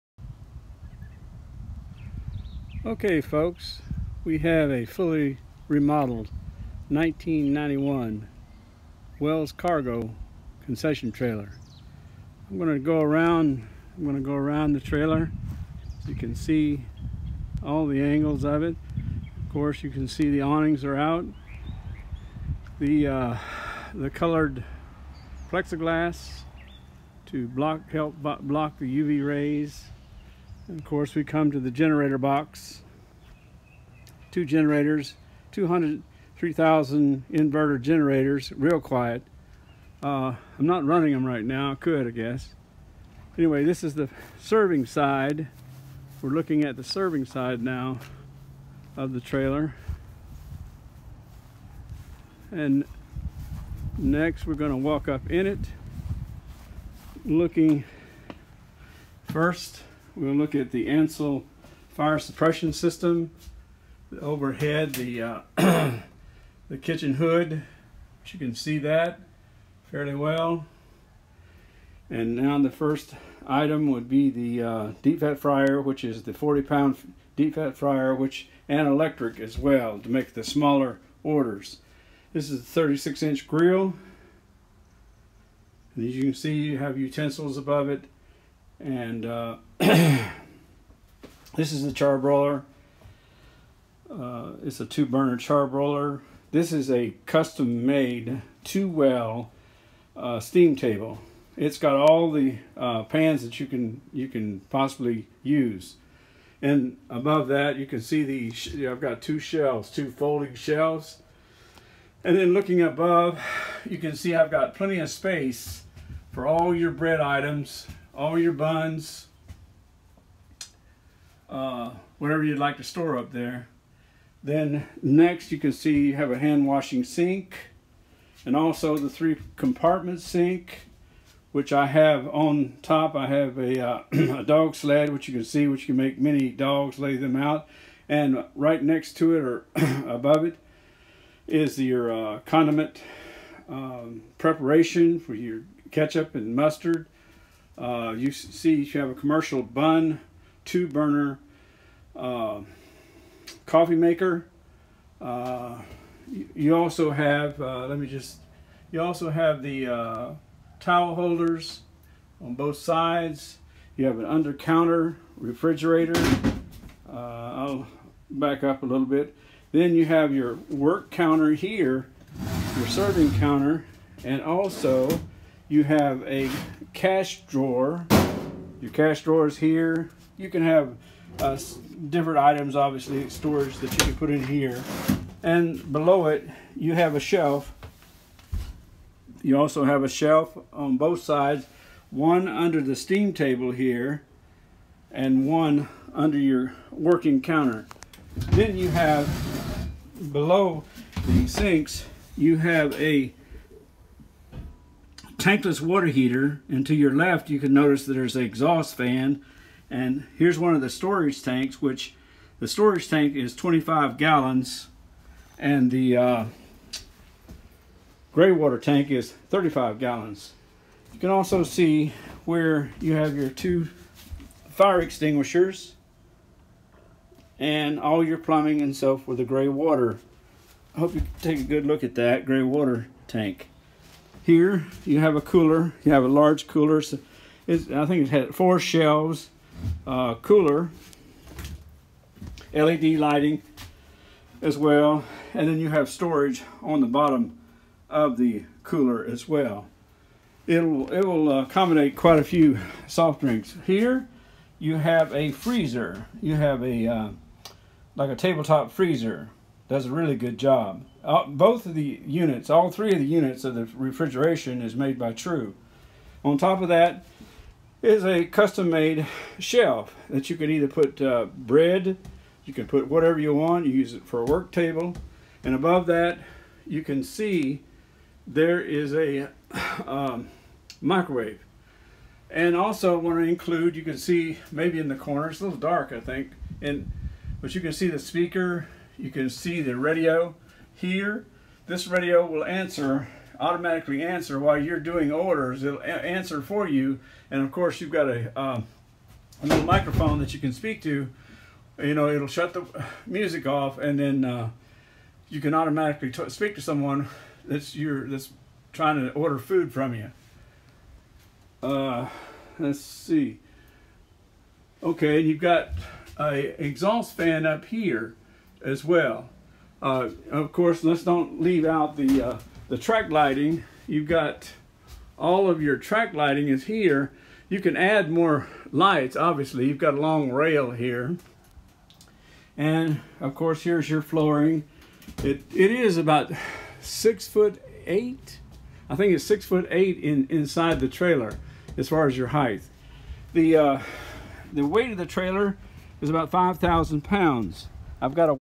1991 Wells Cargo Concession Trailer
Description

Description:
Equipment List
Concession Trailer Options
• Concession manual awning windows 3
• Stainless Steel and FRP.090 easy to clean white wall coverings
• Fully insulated walls and overhead
• Exterior removal stainless steel serving counter
Equipment Power Supply
• Propane Tanks (4) 30 lb LPG
• Generator: 2 Quiet Honda 3000 inverter generators
• Shore Heavy Duty Power Cord 25’
Refrigeration
• Upright Freezer:
True Under Counter Freezer
• Reach-In Upright Cooler:
True 3 Shelf Drink Cooler
• Refrigerator:
True under counter refrigerator
Cooking Equipment
• Charbroil Grill: 18” x 13”
• Flat griddle: 36”
• Fryer(s):
Imperial 40 lb LPG 2 basket fryer:
Krups 4.5 L 2 basket Electric Deep Fryer with preset options:
• Microwave:
Commercial Sharp Microwave Oven
• Steam Table:
Custom Made Full Sized 2 Well Steam Table with Hot Dog,Chili and Bun Holding Pans
Food Service Equipment
• Coffee Machine:
• Commercial Bunn 2 pot warmer coffee maker
• Full Condiment Station with Mustard and Ketchup Pumps
• Stainless Steel Dog Sled
• Kitchen Utensils with Hanger
Kitchen Equipment
• Complete 5’ x 10” Hood with Filters
• Exhaust Fan:
• Commercial Updraft fan, 2 exhaust fans
• Fire Extinguisher:
2 Fire Extinguishers
• Pro Fire Suppression System:
Ansul Chemical fire Suppression System
• Additional S/S work counters
• Carbon Monoxide/Gas Monitor
Electrical
• Interior Lighting Track LED
• GFI Outlets 1
• Mix of 4 gang and 2 gang AC outlets 10
• Breaker Panel: 30 Amp
Plumbing
• Hot Water Heater:
Tankless LPG water Heater
• Hand Washing Sink 1
• Triple Sink: 3 Compartment
• Fresh Water Tank: 25 gal
• Grey Water Tank: 35 gal
Retail Equipment
• Cash Register: Cash Drawer
• POS System: Square contactless card Reader